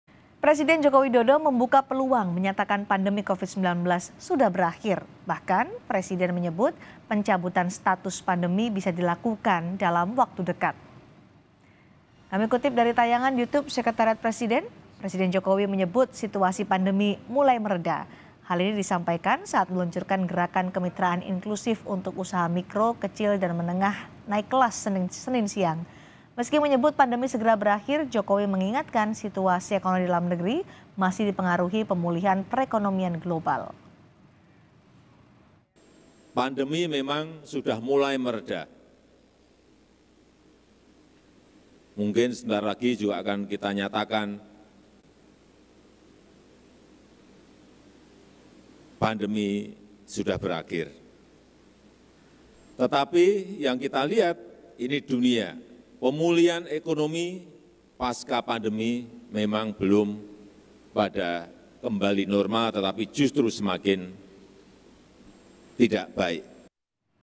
Presiden Jokowi Sebut Pencabutan Status Pandemi Bisa Dilakukan dalam Waktu Dekat!
JAKARTA, KOMPAS.TV - Presiden Joko Widodo membuka peluang, menyatakan pandemi covid-19 sudah berakhir.

Bahkan, Jokowi menyebut pencabutan status pandemi bisa dilakukan dalam waktu dekat.

Baca Juga Jokowi Tidak Mau Komentari soal Anies Baswedan Jadi Capres NasDem: Kita dalam Suasana Duka di

Dikutip dari tayangan Youtube Sekretariat Presiden, Jokowi menyebut situasi pandemi mulai mereda.

Hal ini disampaikan Jokowi saat meluncurkan gerakan kemitraan inklusif untuk usaha mikro, kecil, dan menengah naik kelas, Senin (3/10) siang.

Meski menyebut pandemi segera berakhir, Jokowi mengingatkan situasi ekonomi dalam negeri, masih dipengaruhi pemulihan perekonomian global.